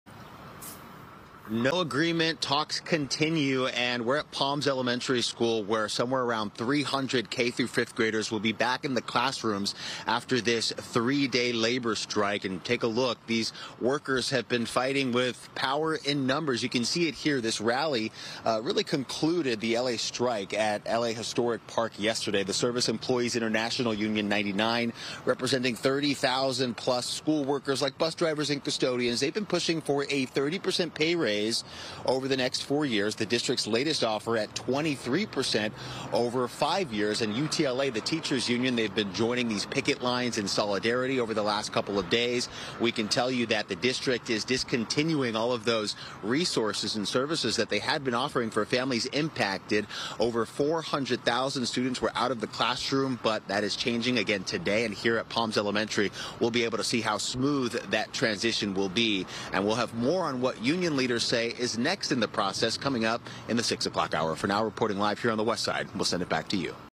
LAUSD schools reopen Friday as 3-day strike ends
No new contract came out of the three-day walkout by members of the Service Employees International Union Local 99 -- nor was there any word on whether progress was being made in the contract dispute between the union and the district.